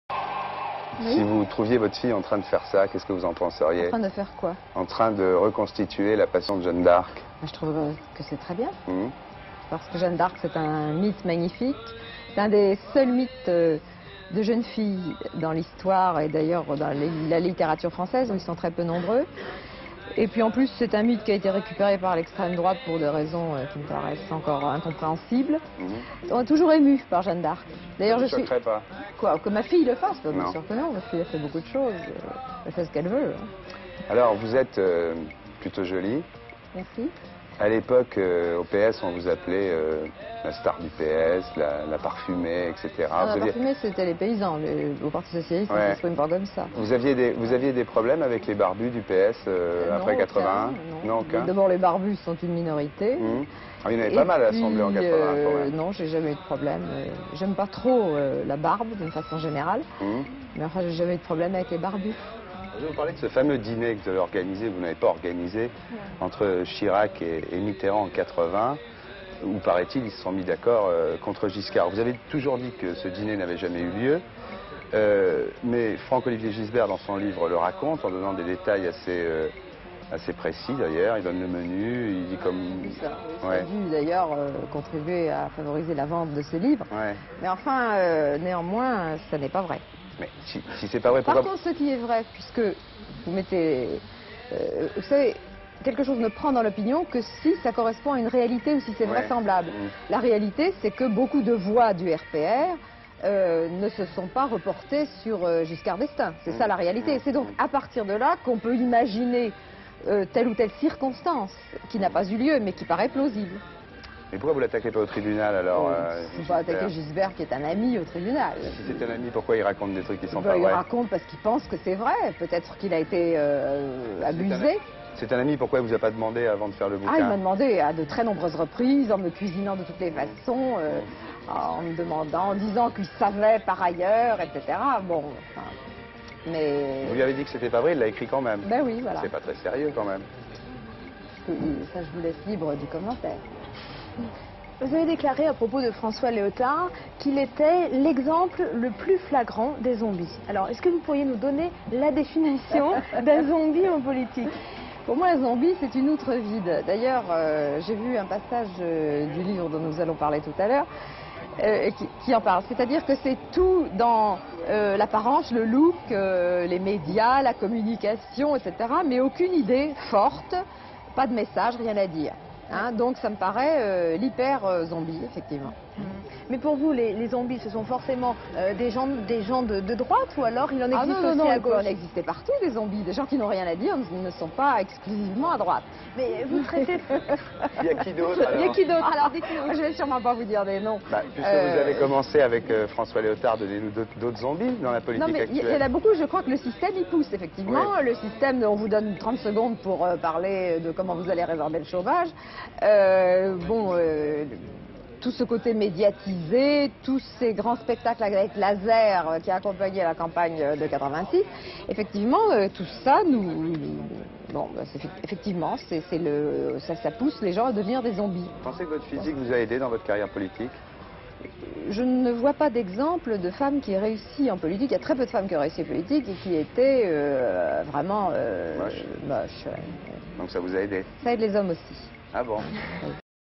Edith Cresson à propos de sa carrière politique - Archive INA
Edith CRESSON évoque ses débuts au parti socialiste, le fameux diner qu'elle aurait organisé entre Jacques Chirac et François Mitterrand en 1980 (qu'elle dément toujours), et François Léotard qu'elle considère comme un zombie. Images d'archive INA